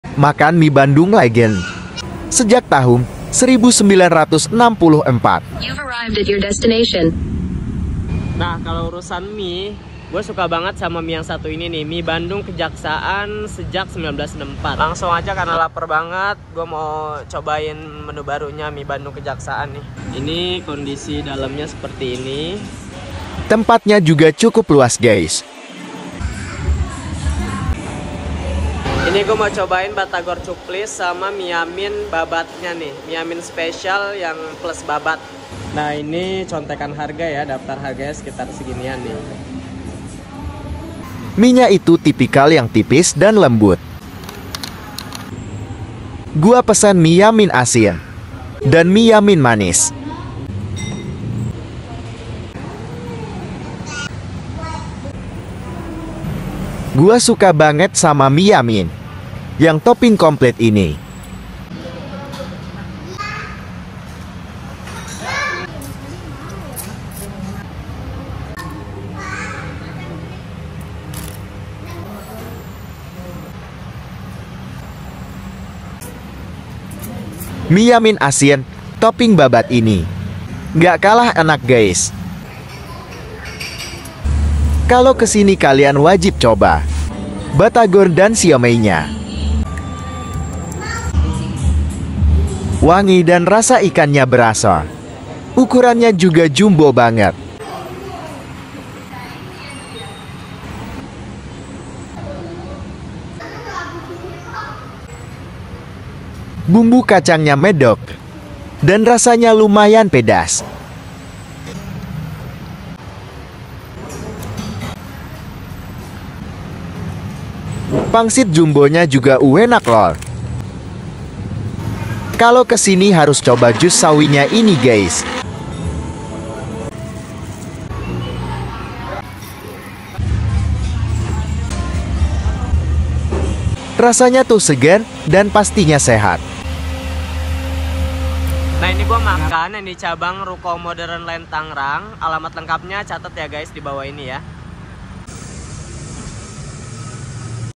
MIE BANDUNG KEJAKSAAN SEJAK TAHUN 1964 SUDAH ADA DI MODERNLAND TANGERANG
‼️MIE BANDUNG LEGEND SEJAK 1964 TERNYATA UDAH ADA DI TANGERANG‼️

Ini tuh Best legend mie yang terkenal enaknya sampai cabang ada dimana-mana. Ada di Jabodetabek, makassar, bali, surabaya, bandung, dll.

Selain mie Yamin nya yang juara, yang gua suka mereka sudah bersertifikasi HALAL, pelayanan OK dan kualitas makanannya higienis

Batagor dan Siomay nya juga juara 😋🙌🏻

Kalian buruan cobain!

📍Mie Bandung Kejaksaan 1964  @miebandungkejaksaan1964 @batagorcuplis
Modernland, Ruko Modern Walk Blok MW No. 7, Jl. Hartono Raya, RT.001/RW006, Klp. Indah, Kec. Tangerang, Banten

📍Jabodetabek, makassar, bali, surabaya, bandung, dll. (lokasi check instagram @miebandungkejaksaan1964)

💰Harga: mulai dari 30 ribuan

⏰ Jam Operasional:
Buka setiap hari dari jam 07.30 sd 20.30

📞Cara order:
- Datang ke Outlet
- Go-Food/Grab-Food/Shopee-Food ✅

Follow @laper.melulu untuk info Kuliner enak 😋